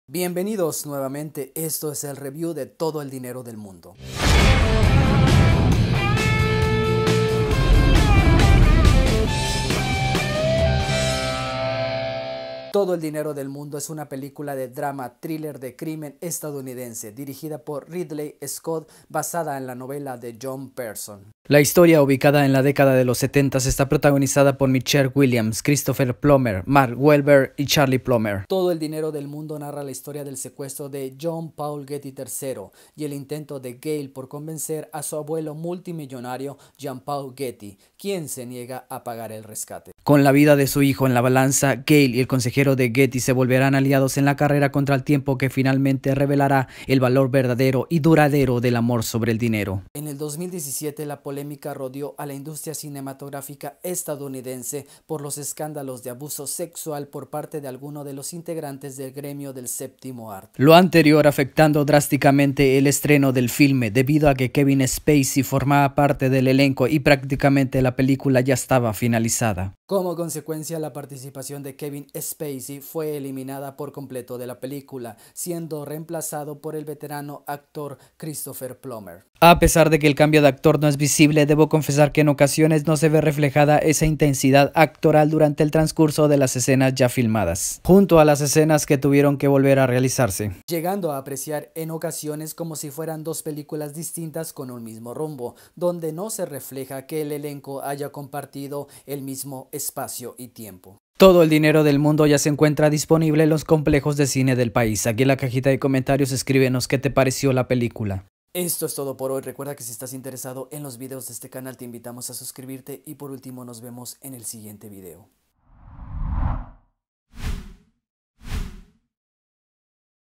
Review: Todo el dinero del mundo • ¿Valió la pena eliminar a Kevin Spacey de la película?
Todo el dinero del mundo es una película de drama thriller de crimen estadounidense dirigida por Ridley Scott, basada en la novela de John Pearson.